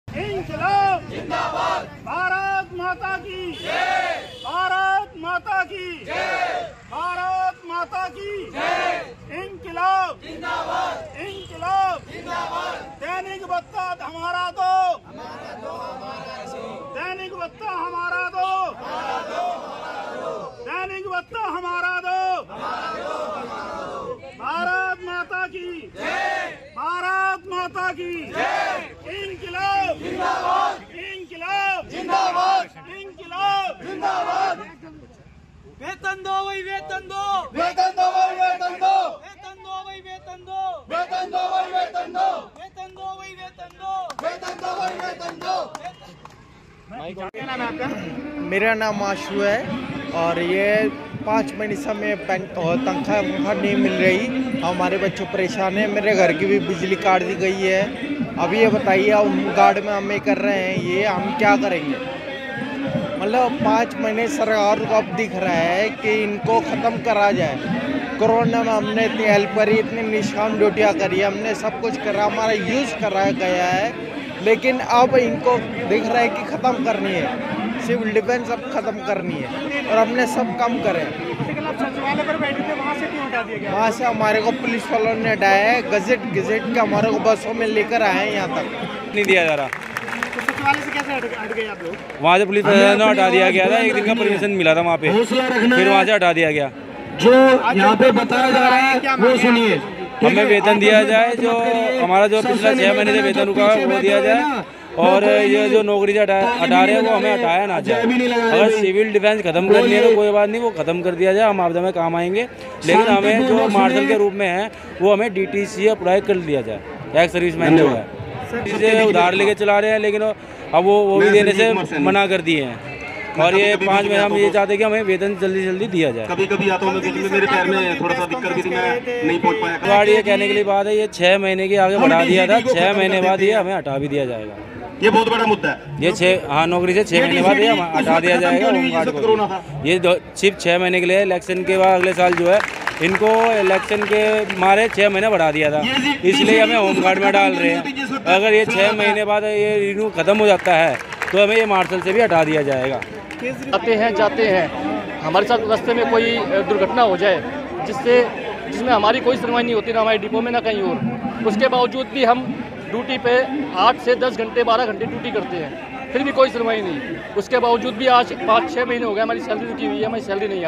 Civil Defense Employees बकाया वेतन की मांग को लेकर फिर से धरने पर बैठे | Delhi News
दिल्ली की DTC की बसों में मार्शल के पदों पर तैनात सिविल डिफेंस के कर्मचारियों ने सोमवार को दिल्ली सचिवालय के बाहर बीच सड़क पर बैठकर सरकार के खिलाफ जम कर नारेबाजी की कर्मचारियों का कहना है कि पिछले कई महीनों से वेतन नहीं मिला है जिसकी वजह से घर का गुजारा करना भी मुश्किल हो रहा है.